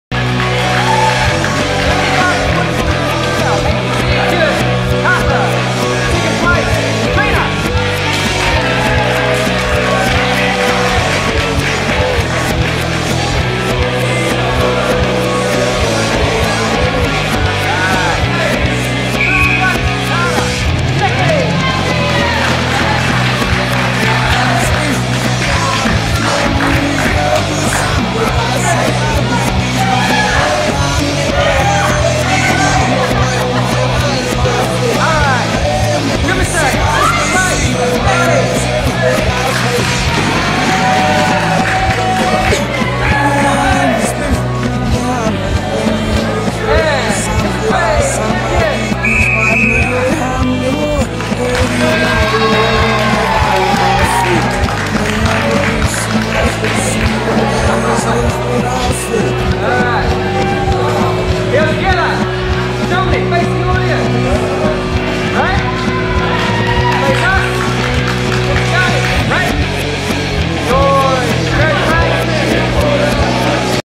gkr wgtn r2 2016 08 28 rg3 35 44yrs female 8th 7th medals a
GKR Karate
Wellington, New Zealand
Round 2
Aotea College
Okowai Road, Aotea
Porirua 5024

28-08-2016